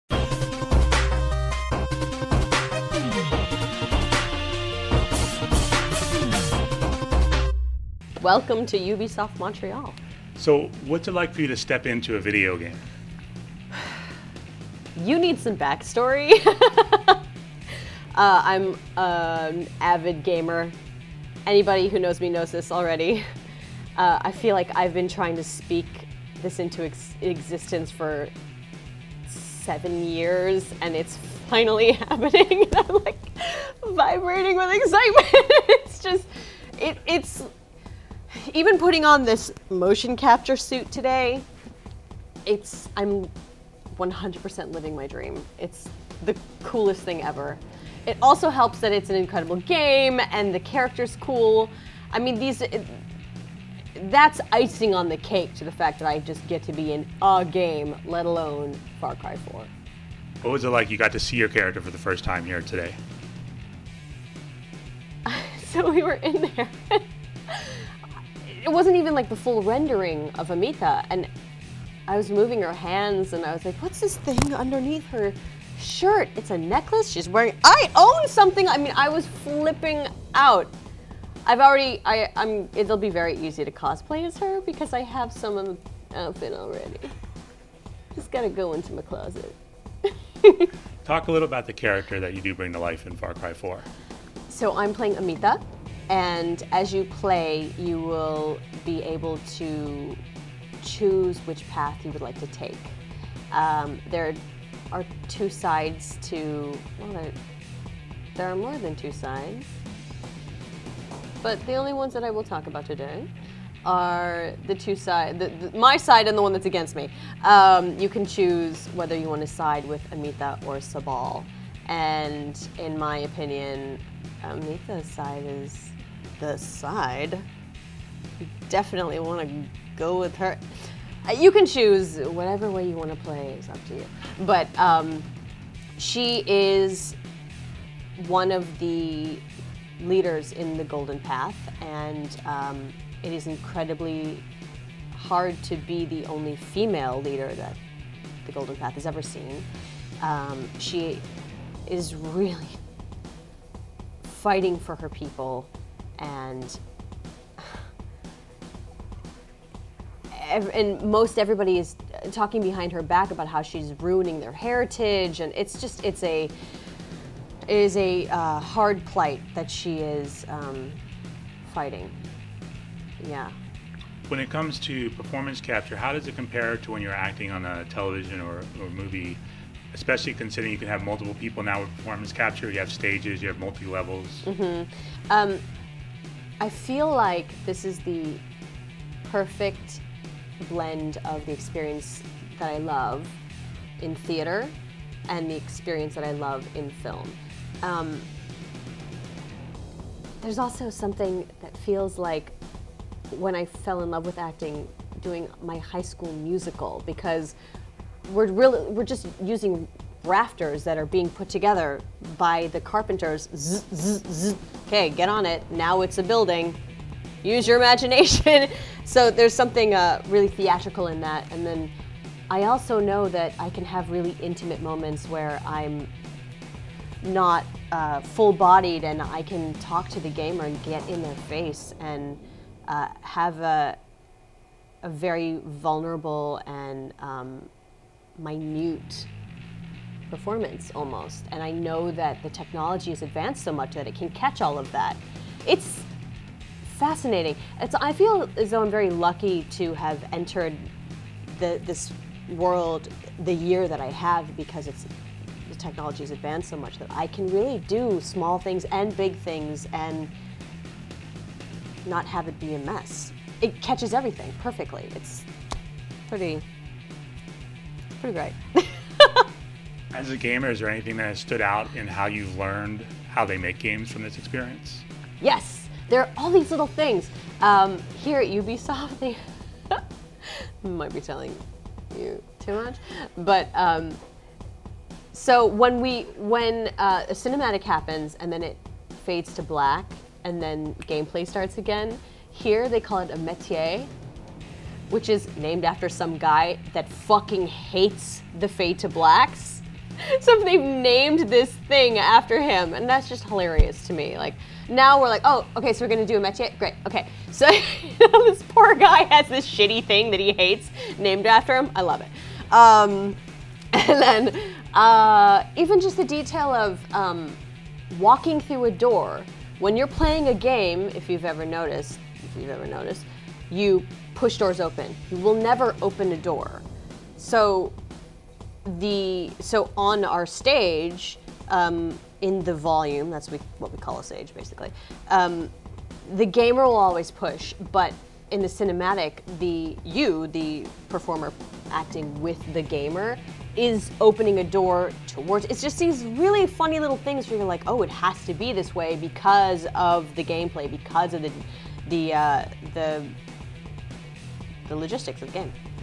Janina Gavankar talks about her role in Far Cry 4
GameCrate caught up with actress and musician Janina Gavankar to talk about her role in the upcoming Far Cry 4.

For more on Far Cry 4, check out GameCrate's full coverage